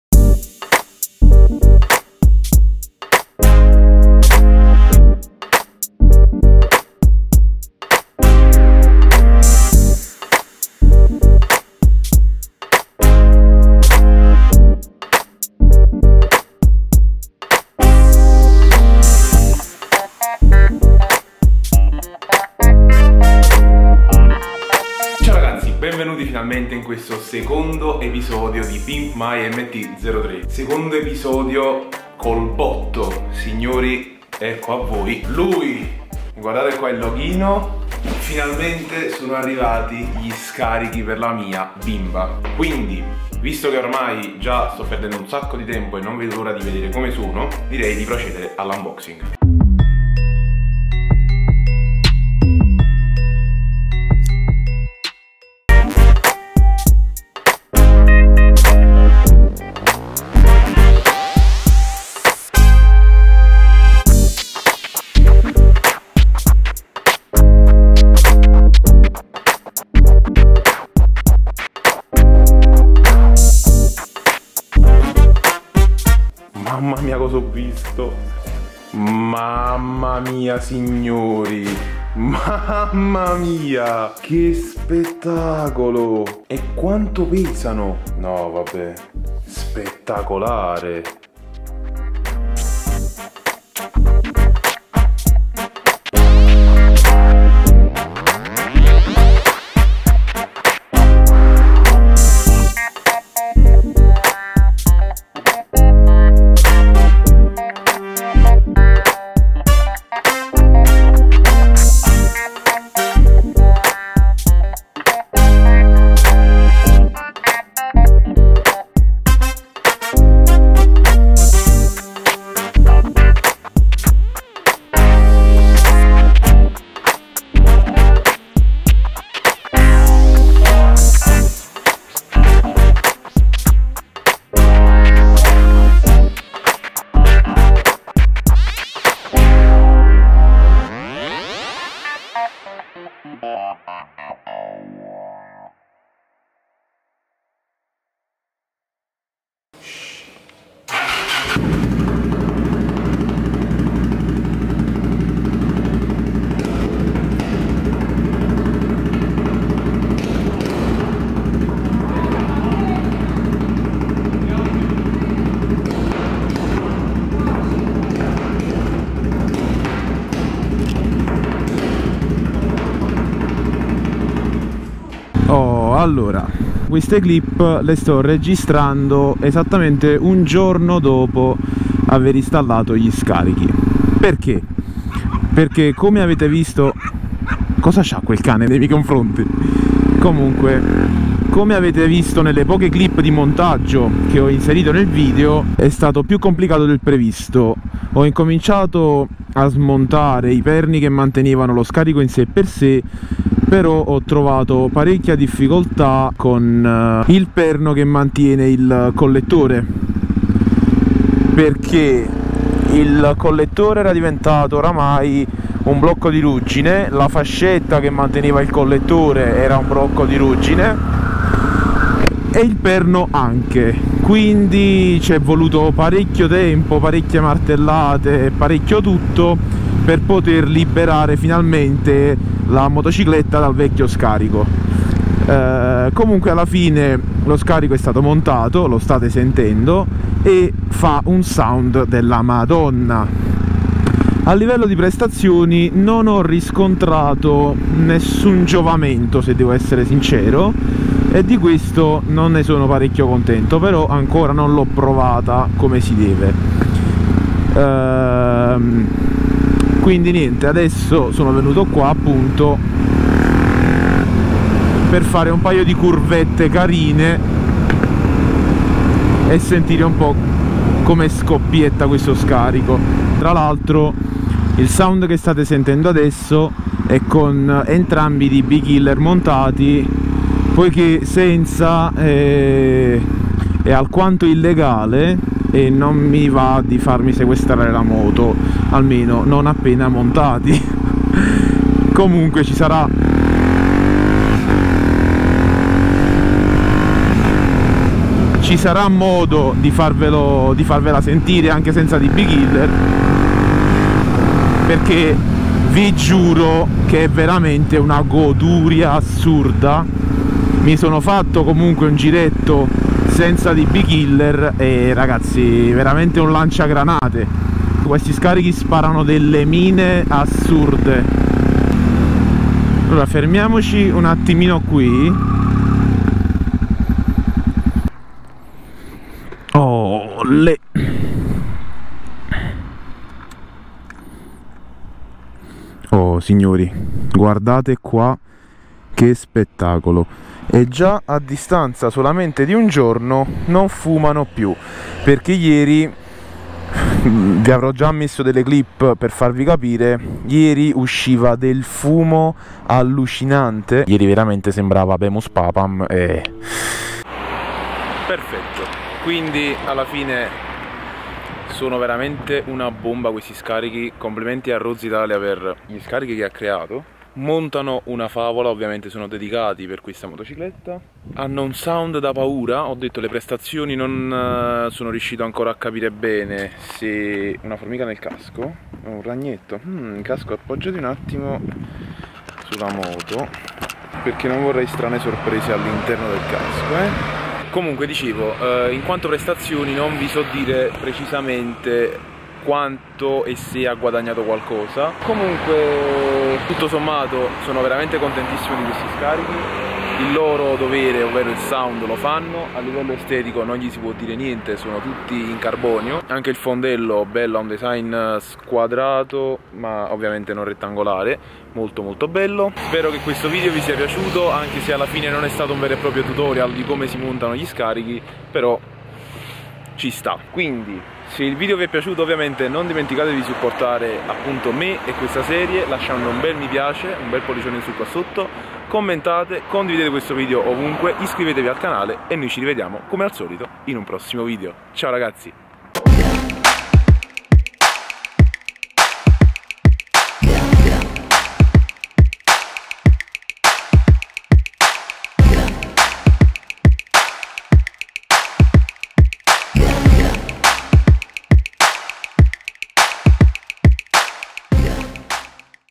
SCARICHI O LANCIAFIAMME? | ROADSITALIA SPECIAL CARBON - PIMP MY MT 03 660 [EP.#2]
Finalmente ho cambiato gli scarichi ad EMME TTINA.
Diciamo che adesso più che degli scarichi monta dei lanciagranate!
Link Utili:
Hardware PC:
- Case PC
- GPU: Pezzo Rarissimo, non si trova più in mercato.
- Tappetino Mouse
- Base di ricarica Wireless
- Pannelli LED